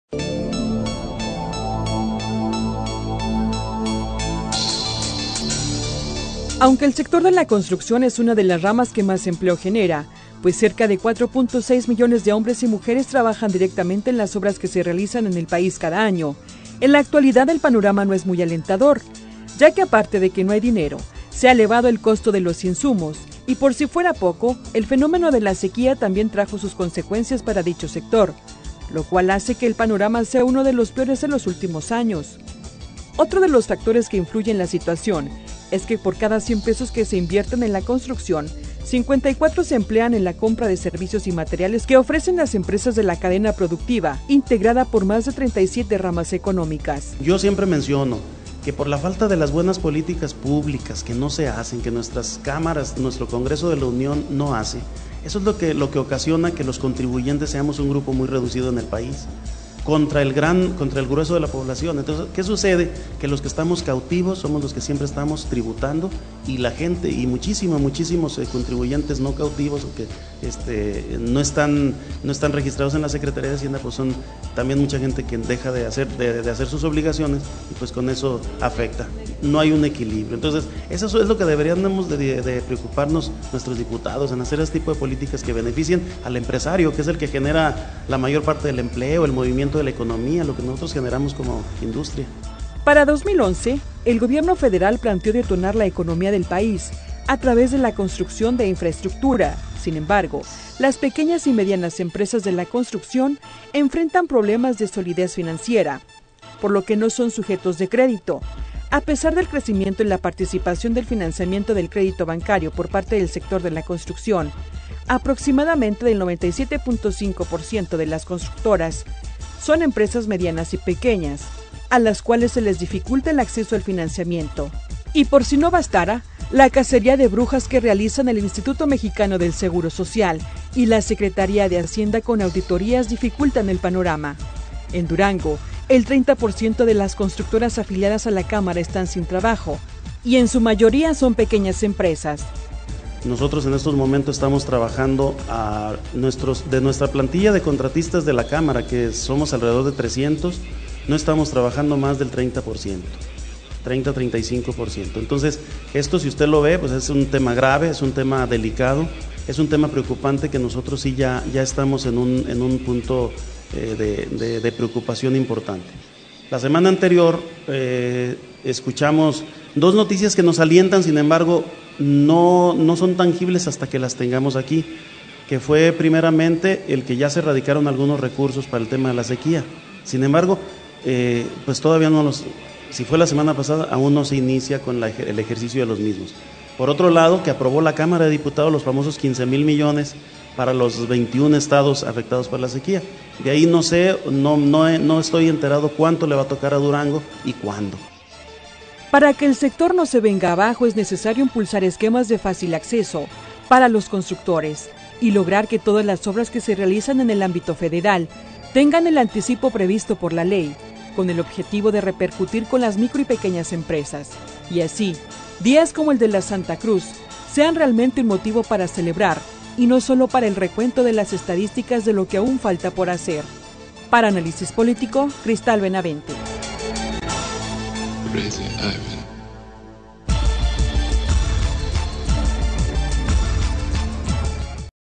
05-05-2012 Industria de la construcción. Bloque 5-9
Cristal Benavente. El sector de la construcción genera empleo a aproximadamente 4.6 millones de hombres y mujeres que trabajan directamente en las obras que se realizan en el país cada año, trabajadores que en buena proporción provienen del campo, la participación del empleo de la construcción en el empleo total es de  7.8%. Por lo cual, la industria de la construcción se convierte para muchos en la puerta de entrada a un proceso productivo más complejo que lo practicado en sus lugares de origen, brindándoles un oficio y el primer contacto con la vida urbana. El jueves 3 de mayo los que trabajan en la industria de la construcción celebraron un día especial, el de la Santa Cruz. Pero más allá del festejo es necesario saber: ¿cuál es el estado de la industria?